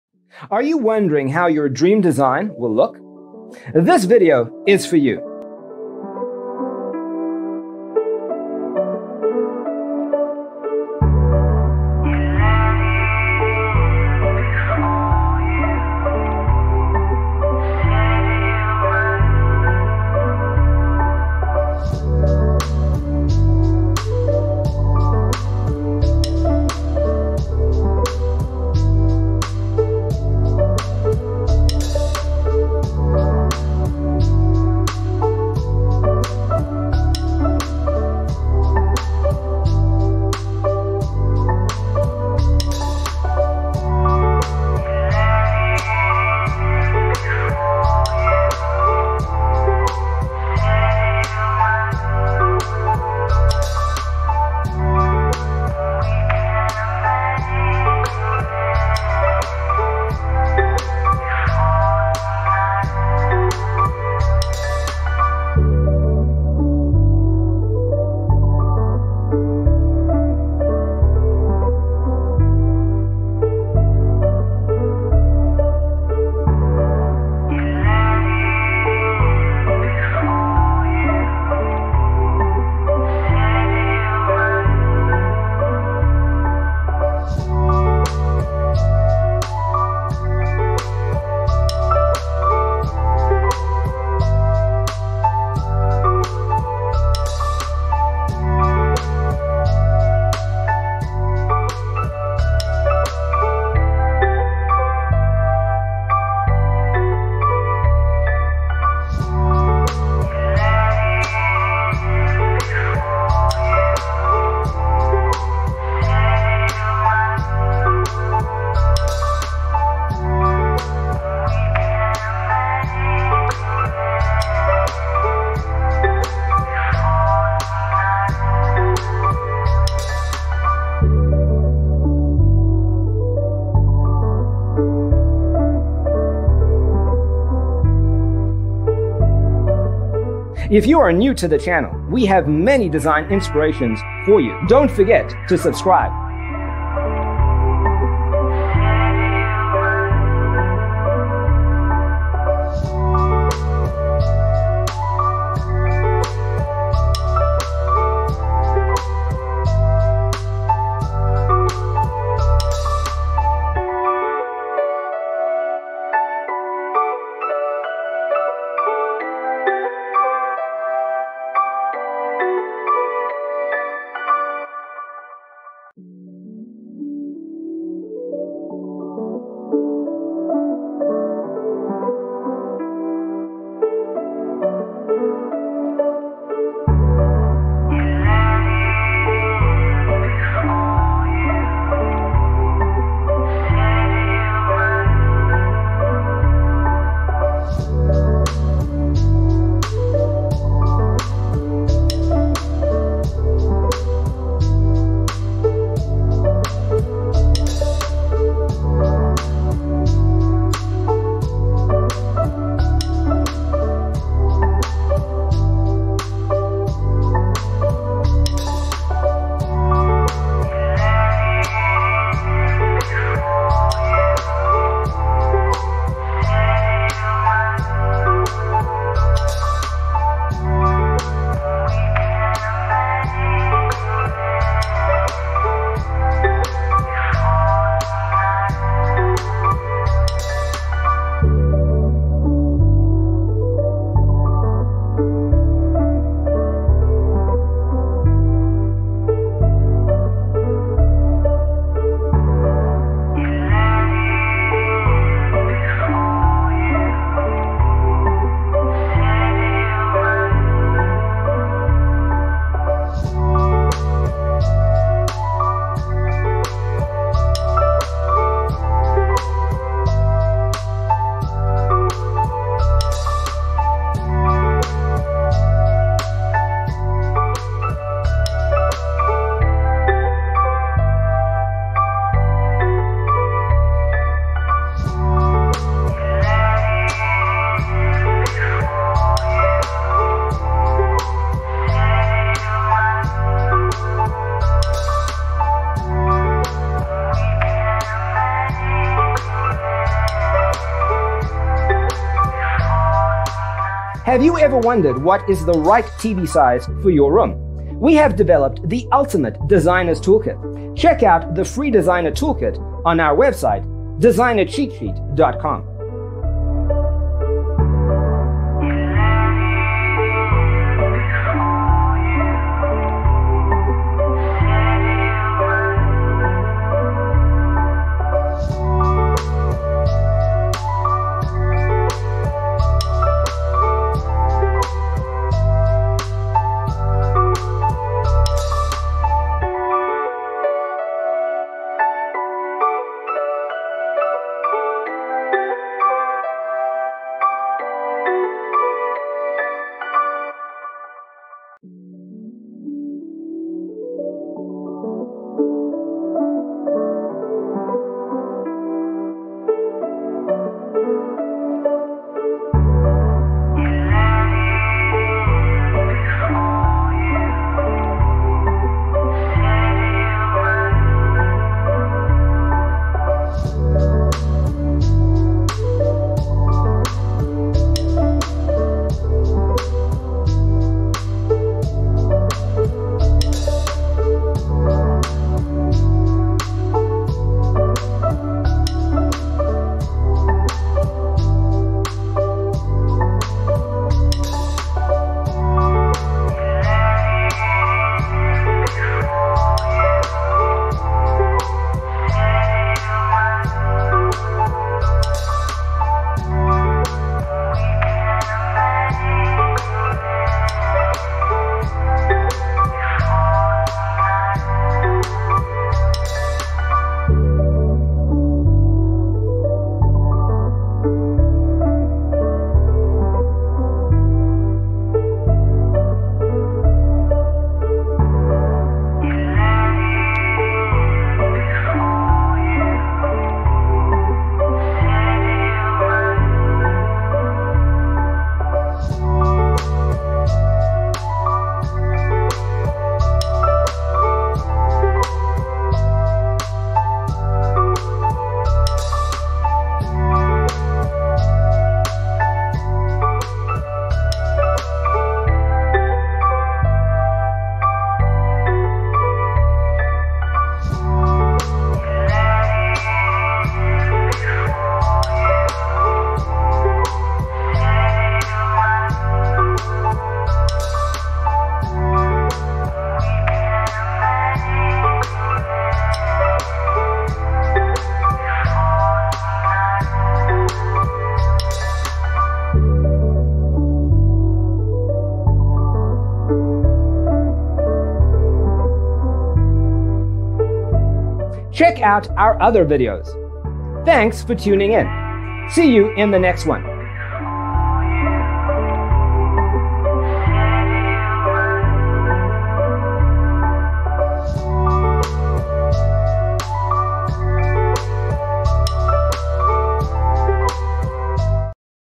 75 Traditional Landscaping For Spring Design Ideas You'll Love ➤
Browse traditional landscapes and gardens. Discover new traditional landscape designs and ideas to boost your homes curb appeal.

0:00:00 - Intro
0:00:12 - Traditional Landscaping For Spring Design Ideas You'll Love: Inspirations 1 to 25
0:03:01 - Traditional Landscaping For Spring Design Ideas You'll Love: Inspirations 26 to 50
0:06:02 - Traditional Landscaping For Spring Design Ideas You'll Love: Inspirations 51 to 75

👋 Well Hello!